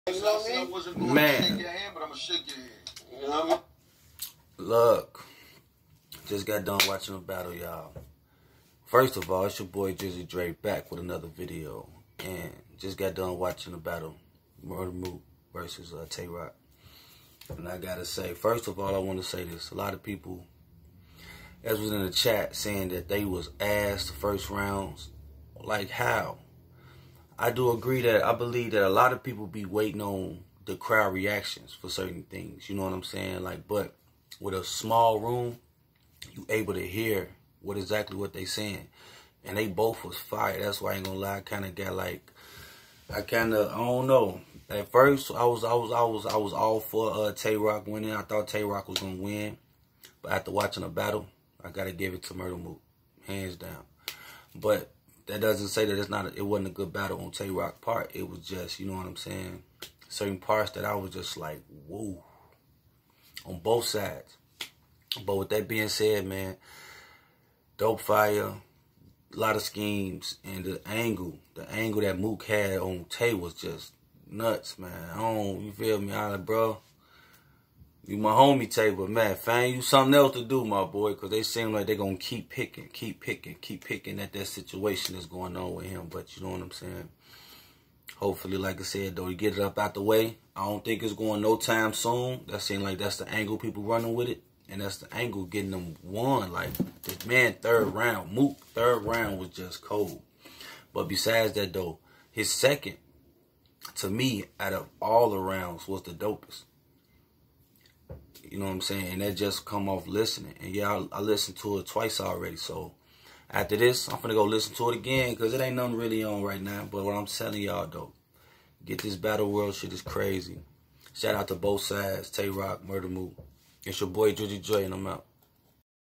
MURDA MOOK VS TAY ROC 🔥🔥🔥🔥🗣💨 I GOT MOOK 2-1 HERE'S WHY
mook vs roc and my reaction